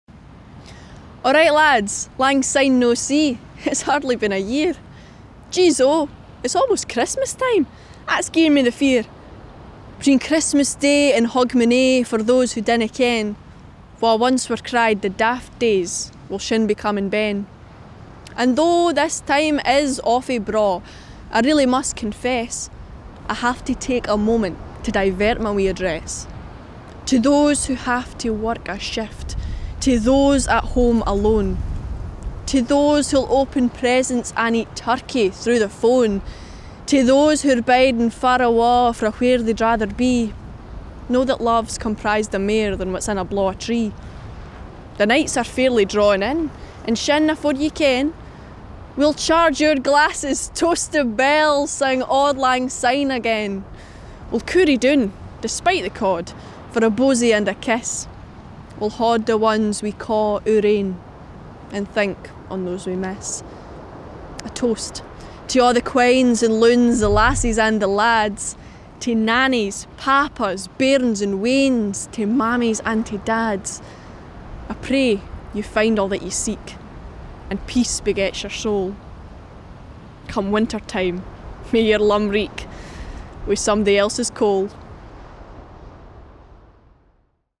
A Toast Tae The Daft Days- a poyum by Len Pennie for LIDL
Lidl asked me tae write a festive wee poyum in Scots tae tell the story of the Daft Days. This is the time between Xmas day and Hogmanay- the original 12 days of Christmas. It's a poyum for everyone, Scot or not, and I'm so excited tae finally share it wae yous all.
I was inspired by 'The Daft Days' by Robert Fergusson, it was such a privilege tae be asked tae take part in this amazing project. The Scots language is so close tae ma heart and being able tae write and recite in it  has been a truly magical experience. Thanks tae Lidl for supporting the Scots Language and the arts in Scotland.

Love, Lennie x